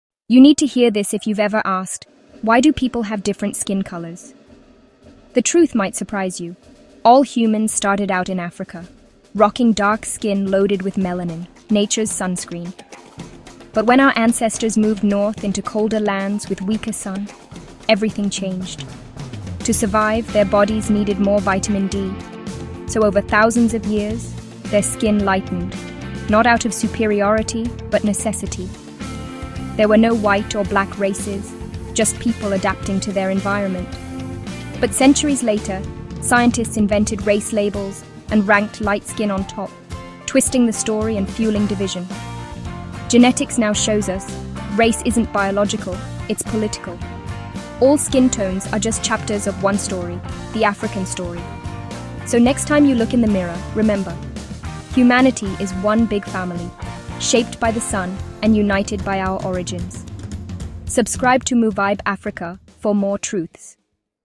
The True Origin of White Skin — Science, History, and the African Beginning
Discover the real science and history behind the origin of white skin — from ancient migrations to genetic adaptation. This short documentary explores how race was invented, how skin color evolved, and why all humanity shares one African rootThe True Origin of White Skin — Science, History, and the African Beginning”